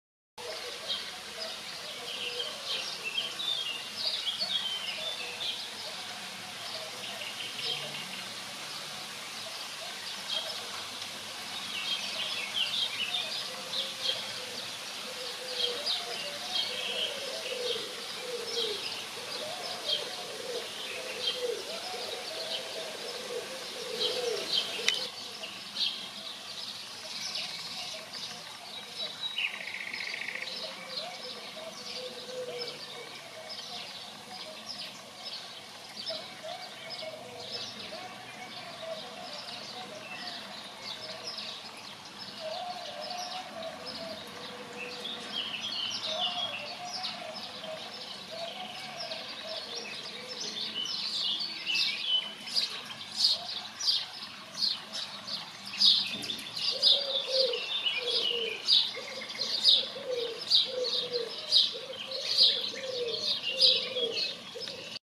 Merle noir dans une flaque d'eau boueuse
Un merle noir qui se nettoie dans une flaque d'eau boueuse. Ca ne va pas le rendre beaucoup plus propre ! :-)